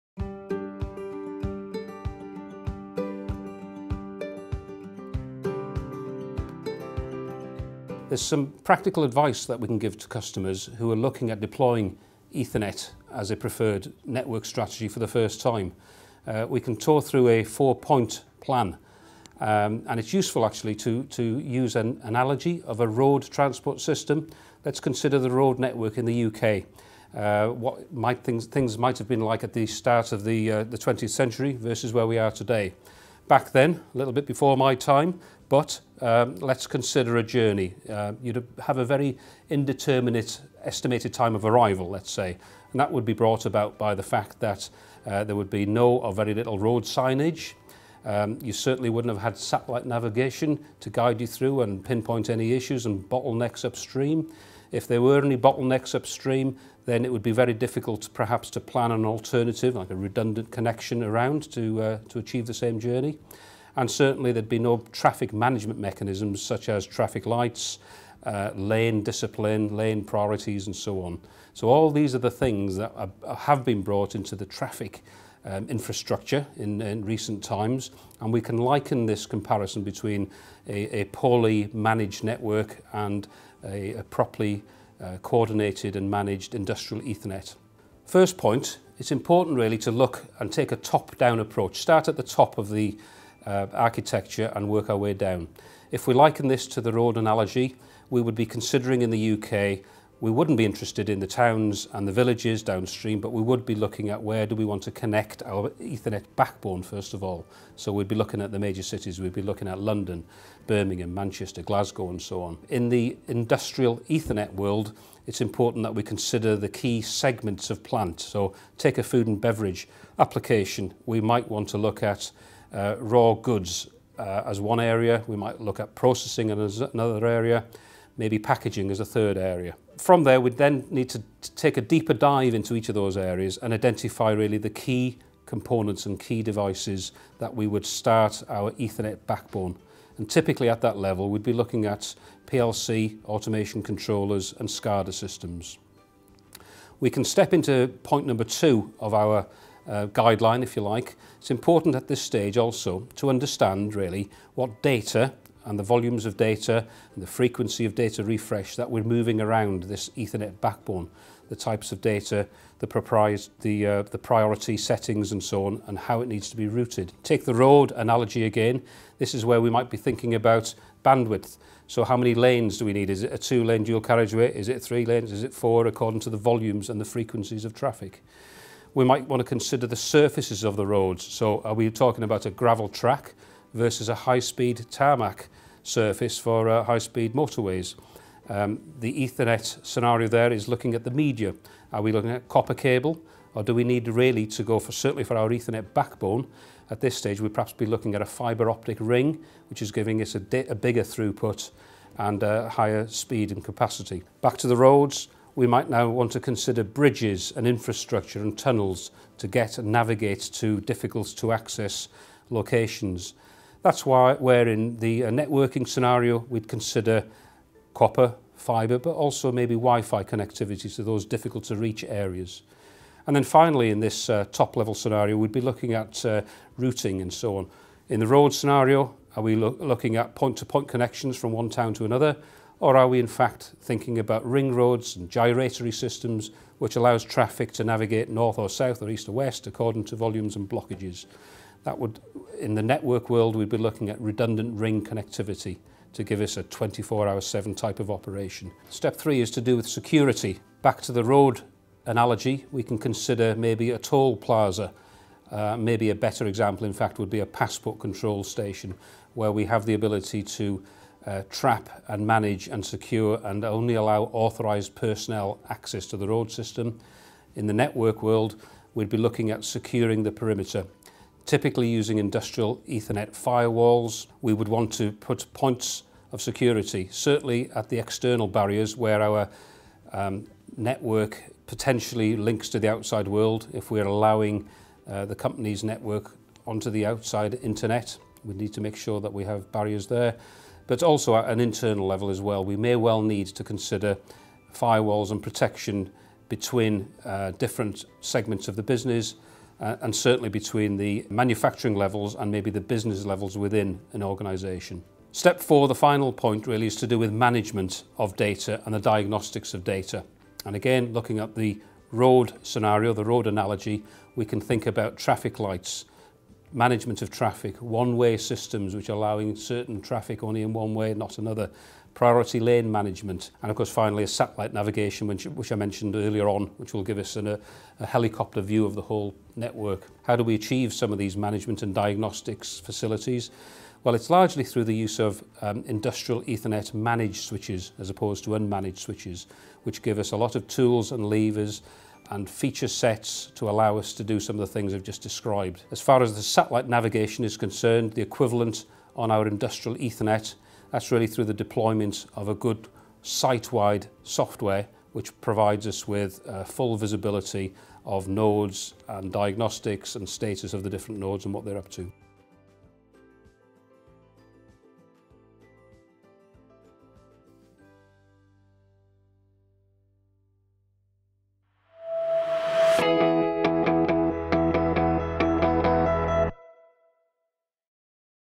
Four Steps to Help Deploy an Ethernet Strategy
Four steps to help deploy an ethernet strategy with Dave Sutton Product Marketing Manager at Schneider Electric.